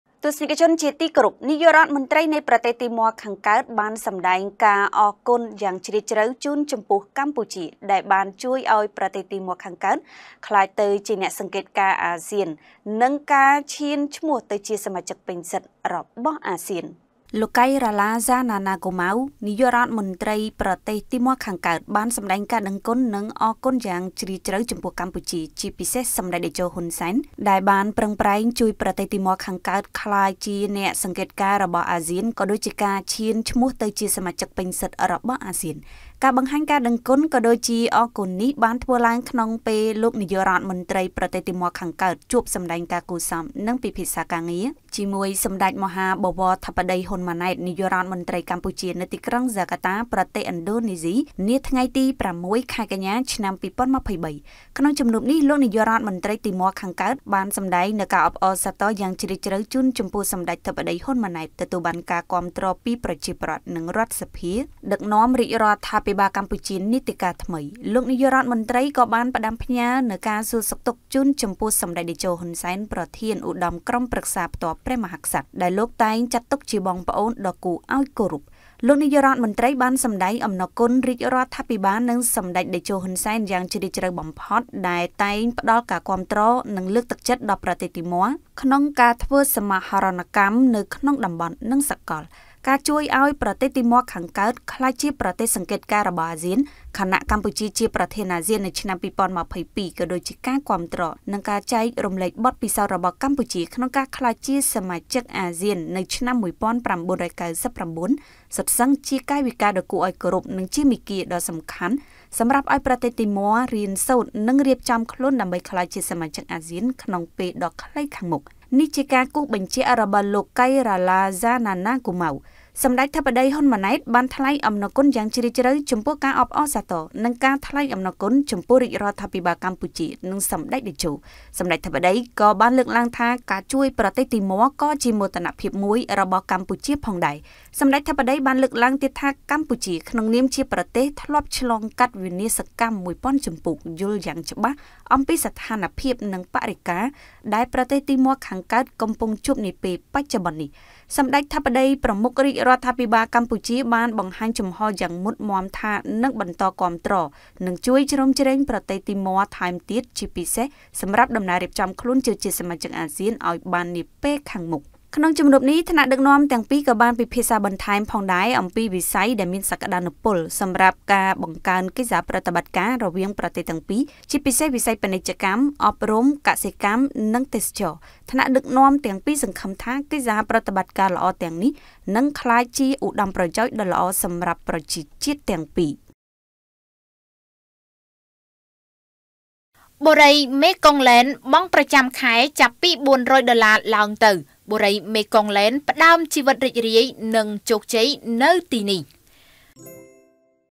នាយករដ្ឋមន្ត្រីទីម័រខាងកើត បង្ហាញការដឹងគុណកម្ពុជា ជួយឱ្យទីម័រខាងកើត
នាយករដ្ឋមន្ត្រីទីម័រខាងកើត បង្ហាញការដឹងគុណកម្ពុជា ជួយឱ្យទីម័រខាងកើតក្លាយ​ជា​អ្នកសង្កេតការណ៍អាស៊ាន